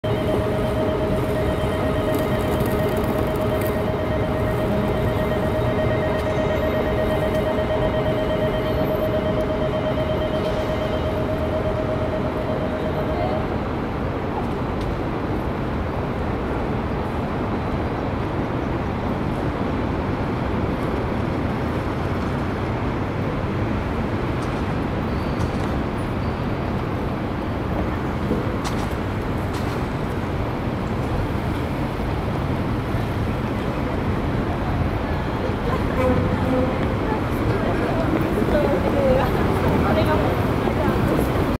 【東京駅前常盤橋プロジェクト】高さ日本一390mの超高層ビル「大手町二丁目常盤橋地区第一種市街地再開発事業」・A棟(212m)の建設状況（2019年7月10日）
■ TOKYO TOKIWABASHI 2027
TOKYO - Tokiwabashi District Redevelopment Project - 390m & 212m & 65m